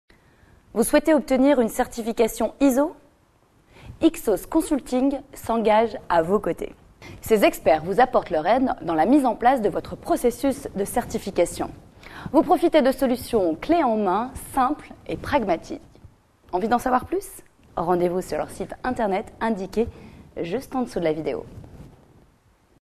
Processus de certification ISO dans le LOIRET
Vous exercez dans le Loiret et souhaitez obtenir une certification ISO ? YCSOS Certification s’engage à vos côtés !
Ses experts vous apportent leur aide dans la mise en place de votre processus de certification…
Ils vous font profiter d’une réponse clés en main à la certification ISO 9001, ISO TS 16949 et à la certification ISO 14001. Ils réalisent également les audits ISO de leurs Clients.
YCSOS vous accompagne tout au long du processus et vous garantit, grâce à ses auditeurs, qui interviennent juste avant l’organisme certificateur, le fait que vous serez certifiés.
Pour les TPE et PME de l’industrie et du service installées dans le Loiret, YCSOS Certification est un installateur et propulseur de certification ISO !

"Aidez Sofian dans son combat contre l'ataxie telangiectasie"